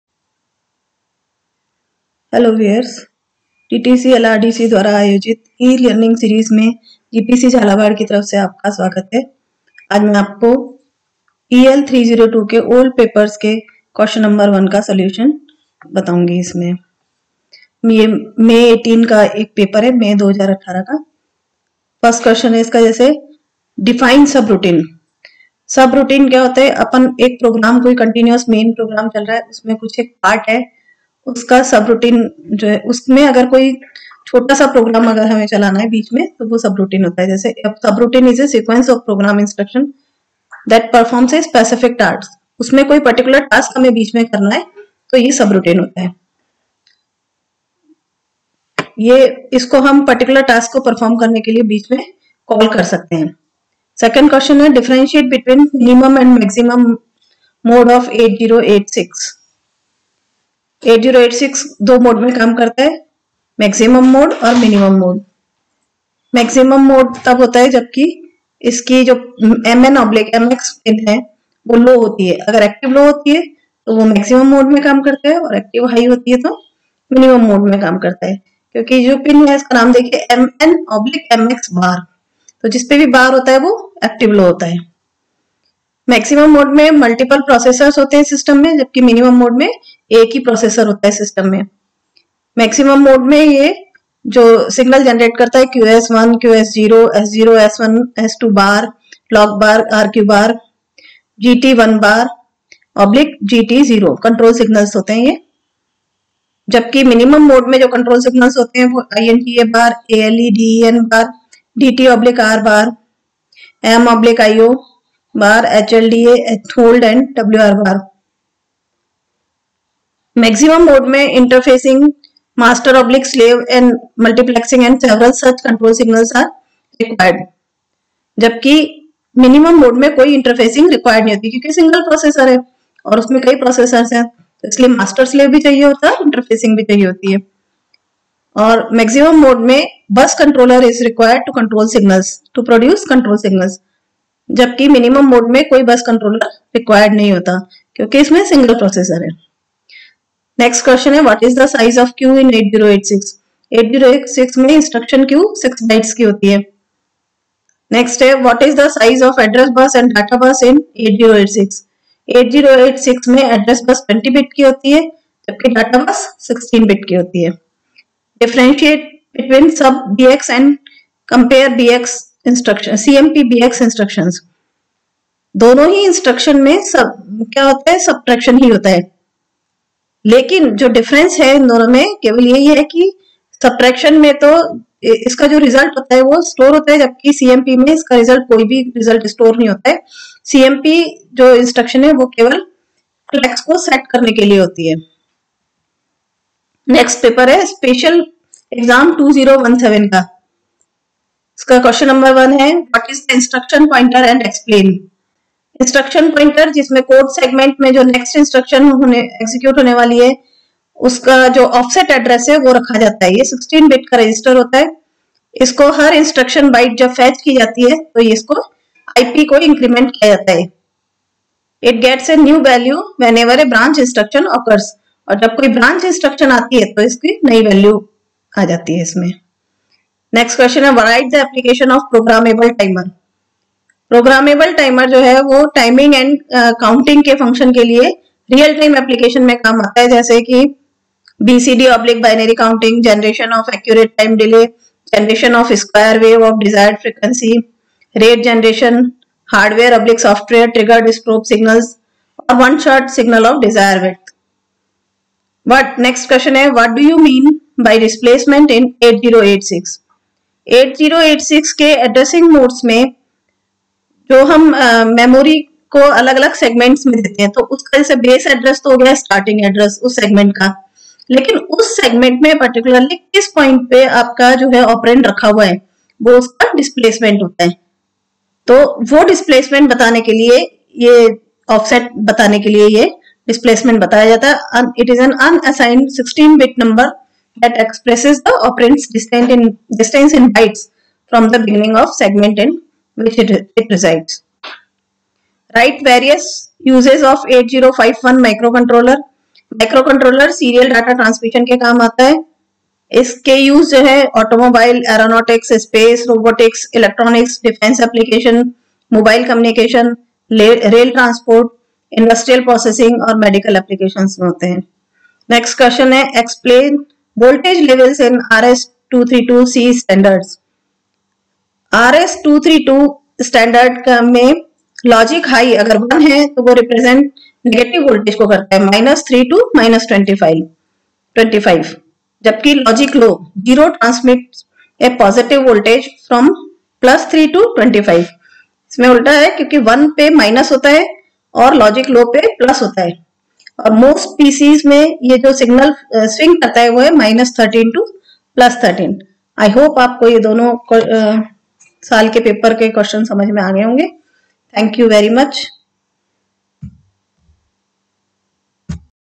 EL-302 ANALYSIS 2 OF OLD PAPERS BY RAJUL GOYAL GPC JHALAWAR
(GPC JHALAWAR)
This video is in accordance with the syllabus of the Polytechnic Colleges of Rajasthan prescribed by BTER